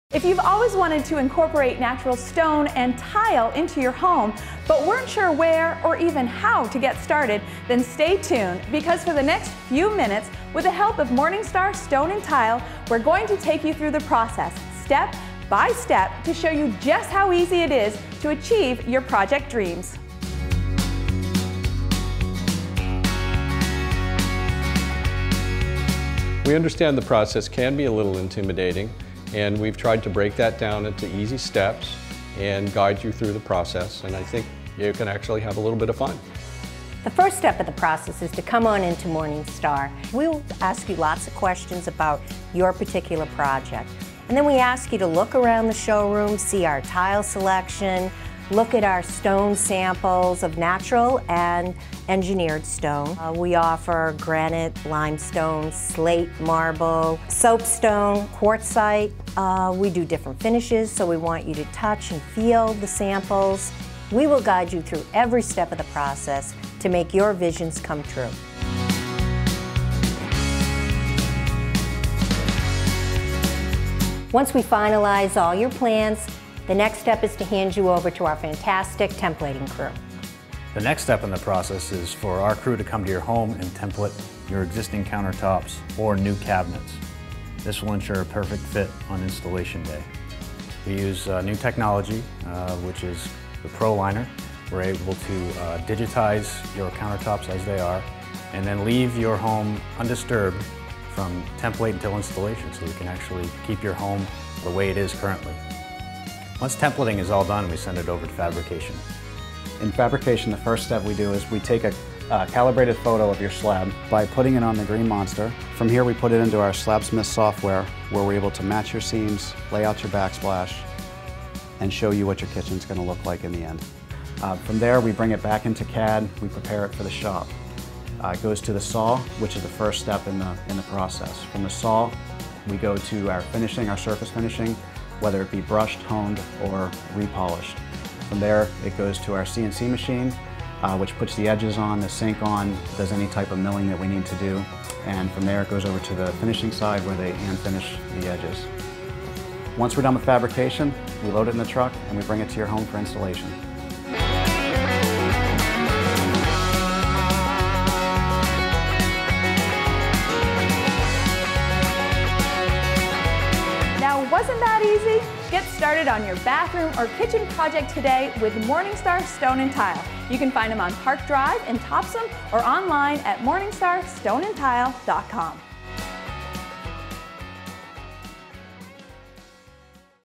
How to Create the Custom Kitchen or Bath Remodel of Your Dreams
Morningstar Stone and Tile, Topsham Maine, describes the process of designing, customizing, and installing custom stone countertops in your kitchen or bath.

Have you always wanted to incorporate natural stone and tile into your home, but not sure where or how to get started? With the help of Morningstar Stone and Tile in Topsham, Buy Local Maine will walk you step by step through the process to show you just how easy it is to achieve the kitchen, bath, or other home remodel project of your dreams.